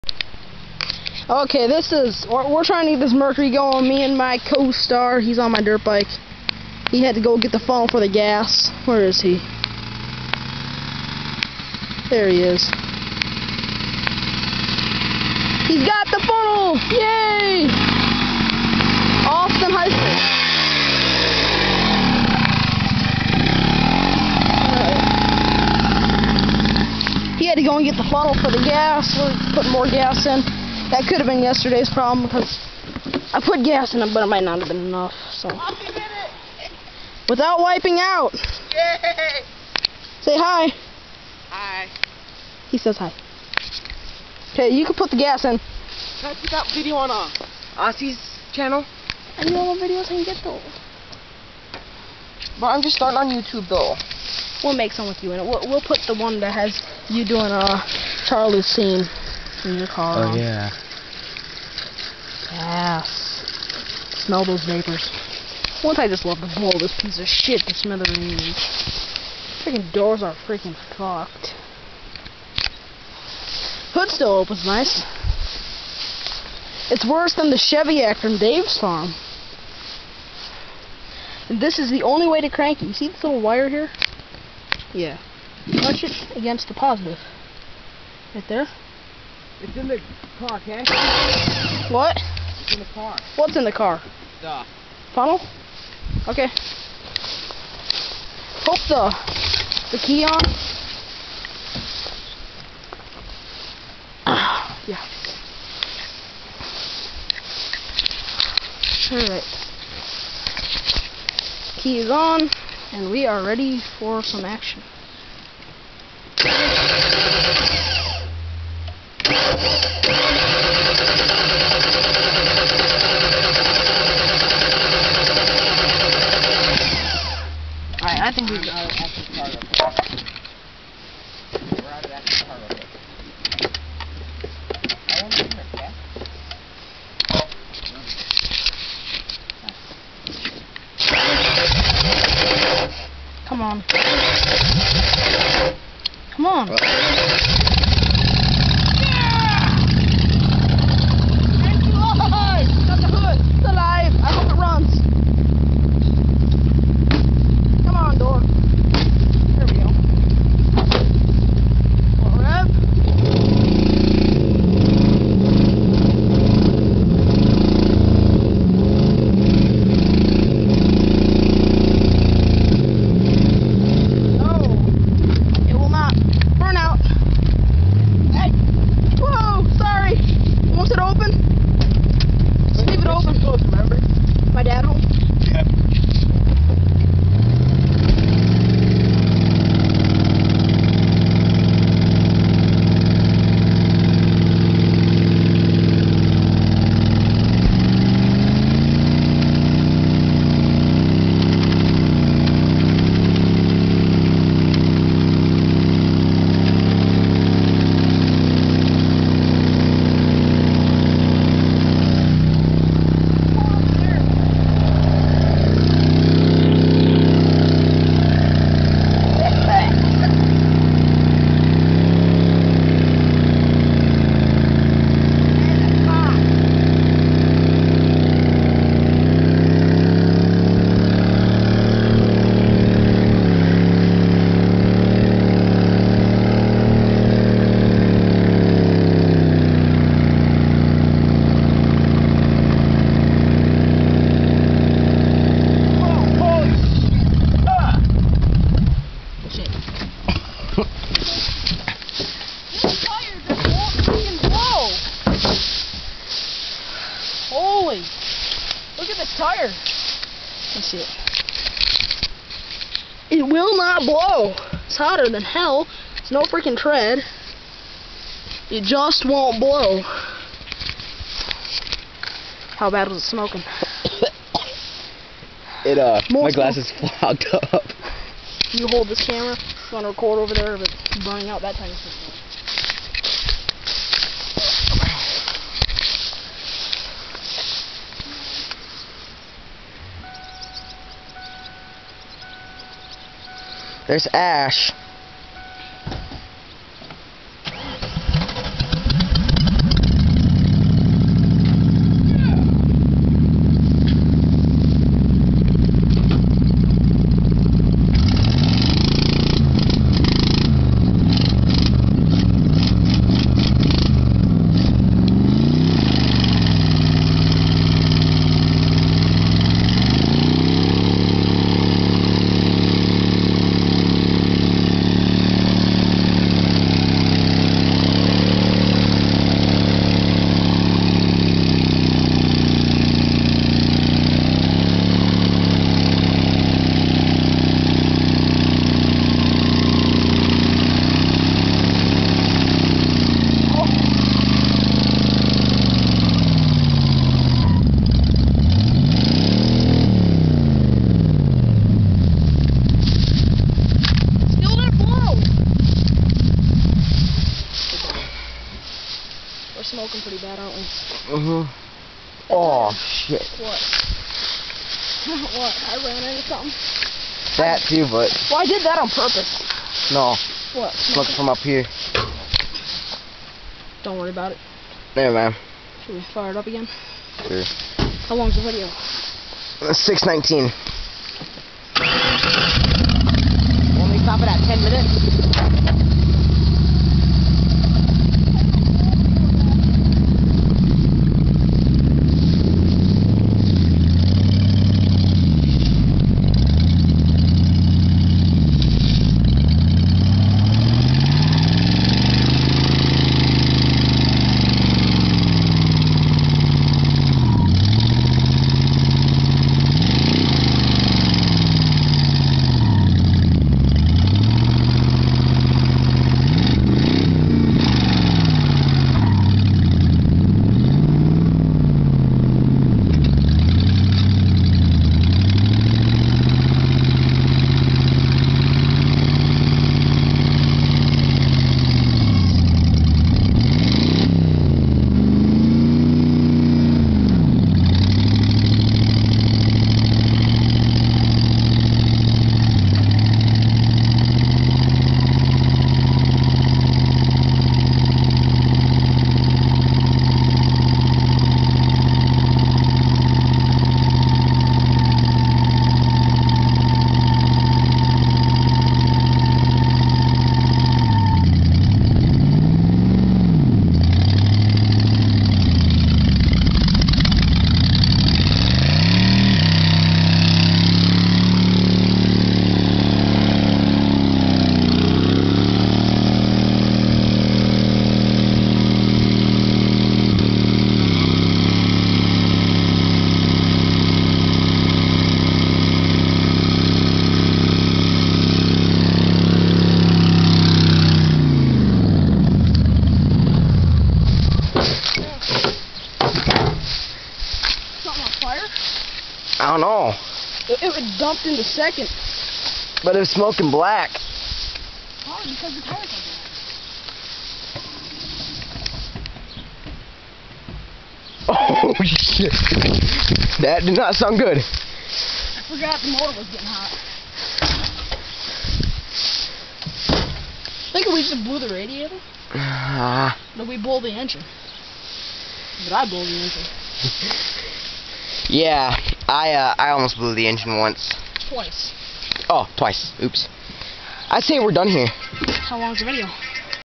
Destroying the Grand marquis
it required more fuel...that fucking tire just wont blow!!!